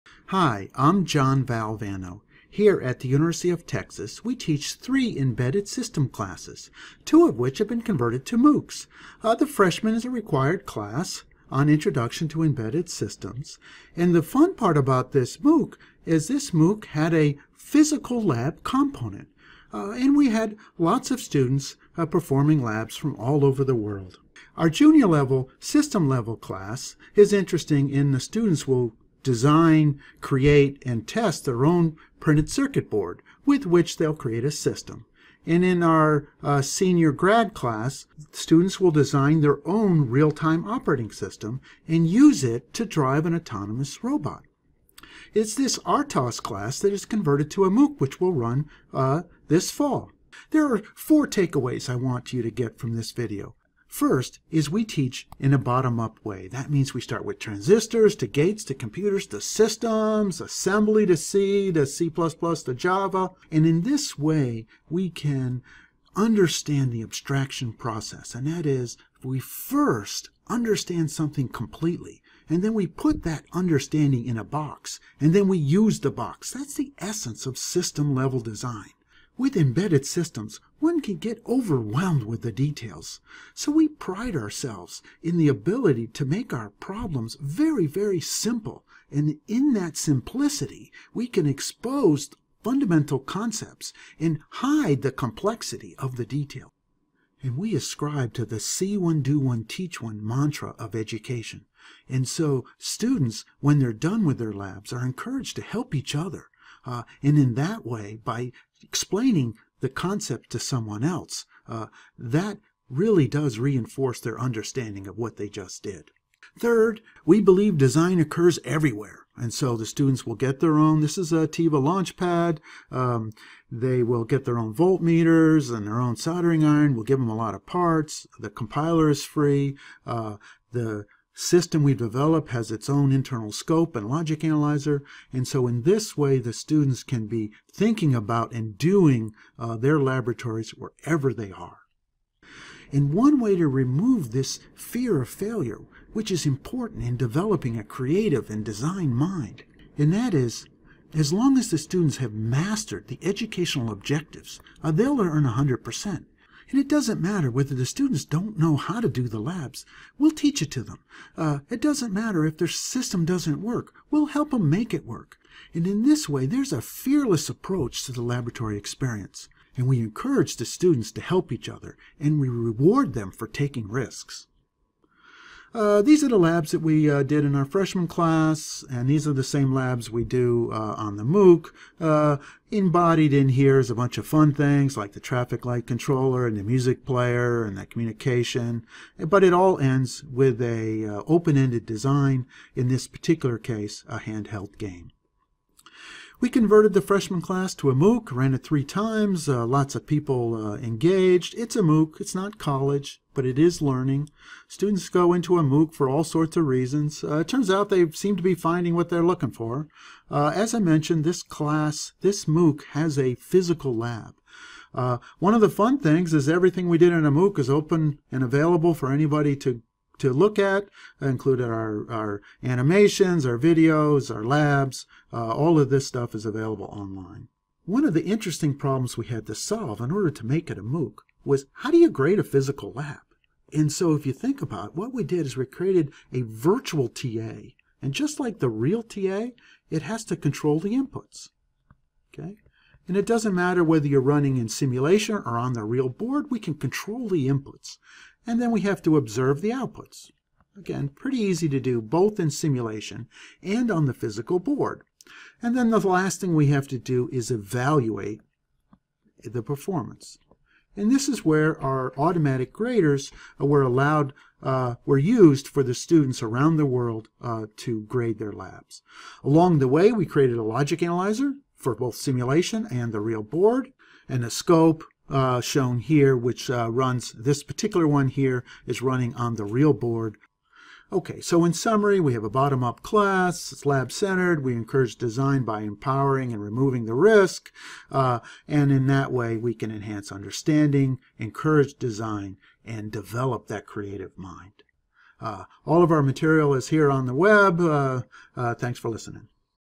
TILaunchPad: Hands on Learning @ Univ of Texas_Valvano_Yerraballi
Eight faculty (Valvano, Yerraballi, Gerstlauer, Bard, Telang, Erez, Janapa Reddi, and Tiwari) teach three embedded system lab courses at the University of Texas at Austin: EE319K, EE445L, and EE445M. We use the TI TM4C123 LaunchPad for all three classes.